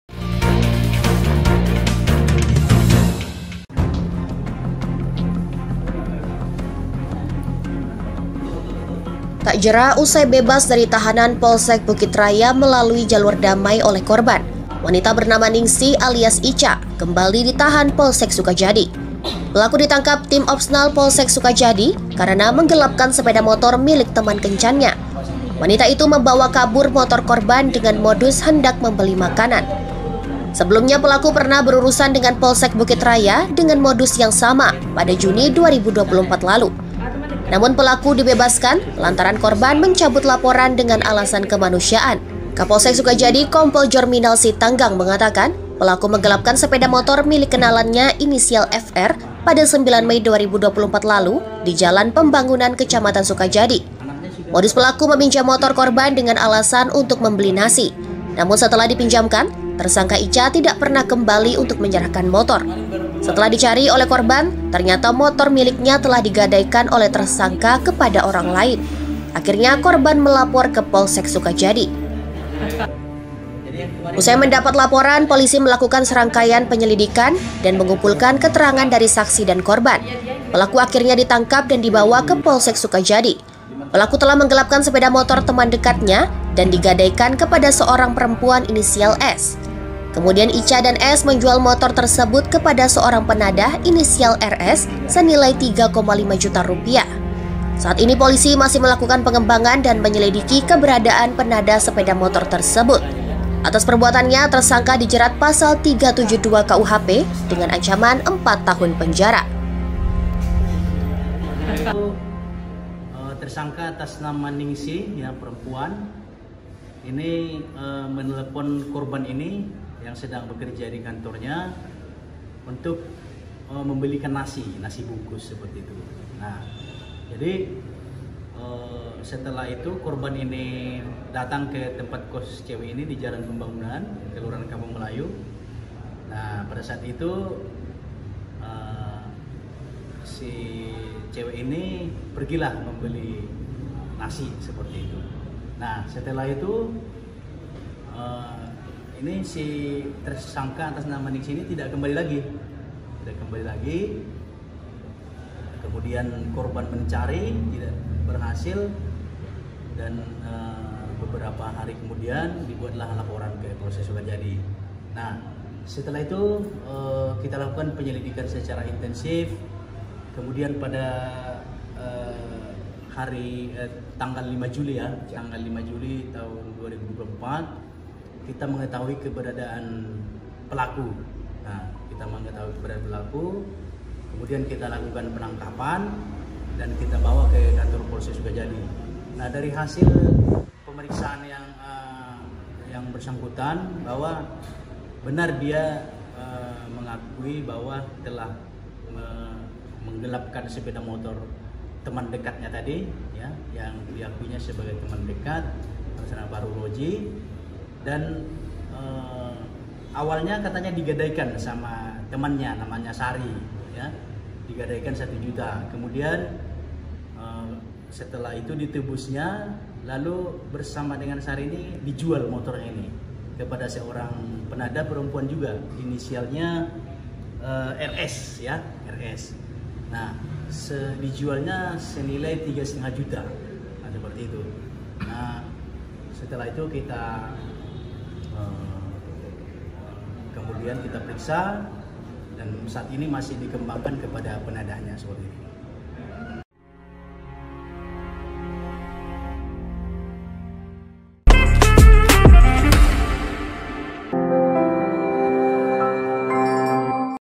Tak Jera, Wanita Ini Kembali Ditangkap Polisi
Tak jera, usai bebas dari tahanan Polsek Bukit Raya melalui jalur damai oleh korban. Wanita bernama Ningsih alias Ica kembali ditahan Polsek Sukajadi.

Pelaku ditangkap Tim Opsnal Polsek Sukajadi karena menggelapkan sepeda motor milik teman kencannya. Wanita itu membawa kabur motor korban dengan modus hendak membeli makanan.

Sebelumnya, pelaku pernah berurusan dengan Polsek Bukit Raya dengan modus yang sama pada Juni 2024 lalu. Namun, pelaku dibebaskan lantaran korban mencabut laporan dengan alasan kemanusian.

Kapolsek Sukajadi, Kompol Nominal Sitanggang mengatakan, pelaku menggelapkan sepeda motor jenis milik kenalannya inisial FR pada 9 Mei 2024 lalu di Jalan Pembangunan, Kecamatan Sukajadi.

Modus pelaku meminjam motor korban dengan alasan untuk membeli nasi. Namun setelah dipinjamkan, tersangka Ica tidak pernah kembali untuk menyerahkan motor. Setelah dicari oleh korban, ternyata motor miliknya telah digadaikan oleh tersangka kepada orang lain. Akhirnya korban melapor ke Polsek Sukajadi.

Usai mendapat laporan, polisi melakukan serangkaian penyelidikan dan mengumpulkan keterangan dari saksi dan korban. Pelaku akhirnya ditangkap dan dibawa ke Polsek Sukajadi.

Pelaku telah menggelapkan sepeda motor teman dekatnya dan digadaikan kepada seorang perempuan inisial S. Kemudian Ica dan S menjual motor tersebut kepada seorang penadah inisial RS senilai Rp3,5 juta.

Saat ini polisi masih melakukan pengembangan dan menyelidiki keberadaan penadah sepeda motor tersebut.

Atas perbuatannya tersangka dijerat pasal 372 KUHP dengan ancaman 4 tahun penjara.